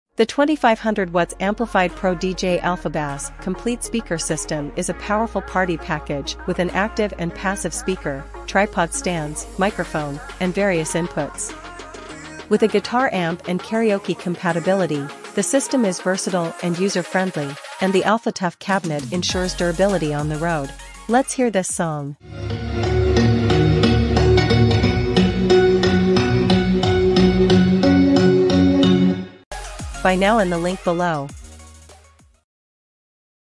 Alphasonik All-in-one 15" Powered 2500W PRO DJ Amplified Loud Speakers
The 15" 2500 Watts Amplified PRO DJ ALPHABASS Complete Speaker System is a party package that includes an active and passive speaker, two tripod stands, a 20-ft STC cable, and a wired microphone. It has built-in Bluetooth, USB/SD card/AUX inputs, FM radio, and an MP3 player.
The system features echo effect control, an Alpha-Q equalizer, and a durable AlphaTough cabinet. It has LED indicators and various inputs, including XLR, RCA audio, banana plug, and 3.5mm auxiliary. The system includes a guitar amp, direct karaoke compatibility, and a high-quality sound output.